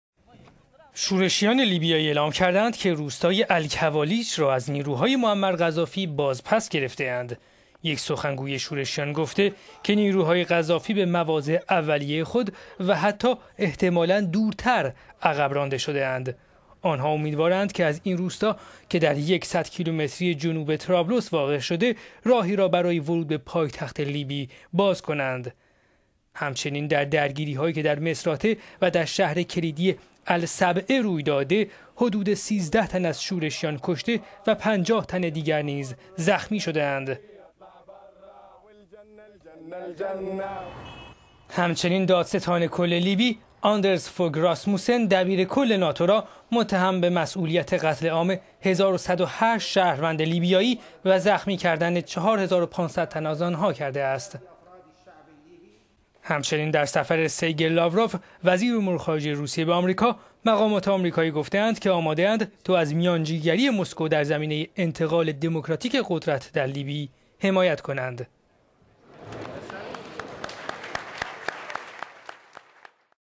دادستان کل لیبی، دبیر کل ناتو...
شورشیان لیبیایی اعلام کردند که روستای القوالش را از نیروهای معمر قذافی باز پس گرفته اند.

یک سخنگوی شورشیان گفته که نیروهای قذافی به مواضع اولیه خود و حتی احتمالا دورتر عقب رانده شده اند.

آنها امیدوارند که از این روستا که در یک صد کیلومتری جنوب طرابلس واقع شده، راهی را برای ورود به پایتخت لیبی باز کنند.